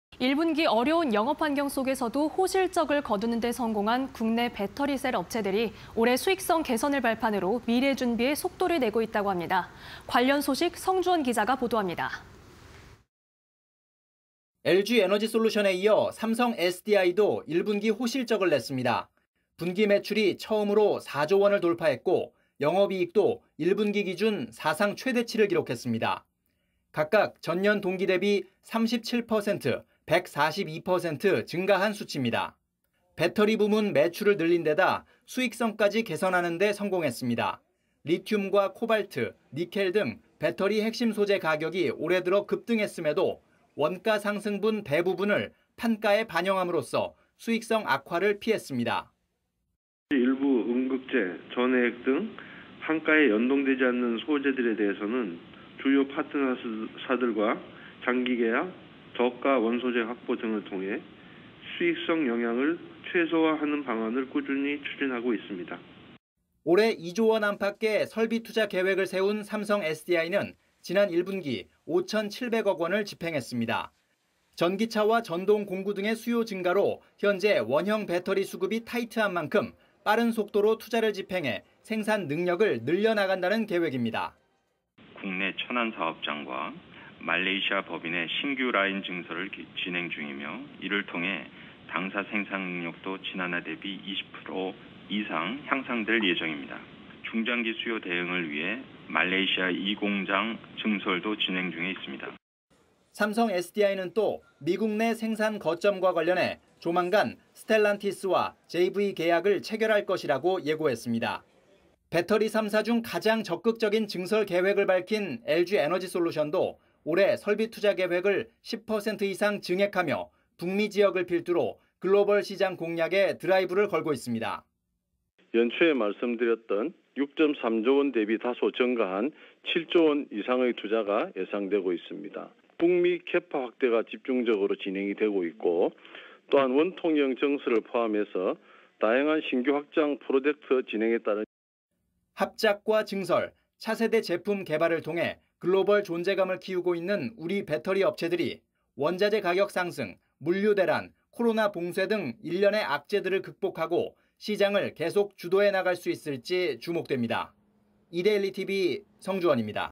악재에 짓눌린 배터리업계... 미래 준비에 '올인' (20220428)
- 리튬 등 핵심소재 가격 급등... "대부분 판가 연동"
- 1분기 설비투자 5696억... "수요 대응 위해 꾸준히 증설"
- 삼성SDI "스텔란티스와 JV 협의 마무리 단계... 조만간 계약"
- LG에너지솔루션, 설비투자 증액... "올해 7조원 이상 투자"